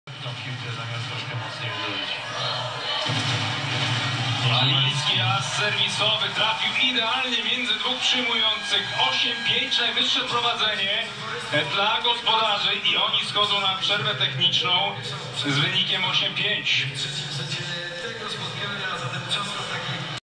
Marcin Waliński Nr 1 - As, PlusLiga 2016
Marcin Waliński Nr 1 (Przyjmujący - Indykpol AZS Olsztyn) wykonuje asa serwisowego w meczu PlusLigi pomiędzy Indykpolem AZS Olsztyn, a Jastrzębskim Węglem - luty 2016.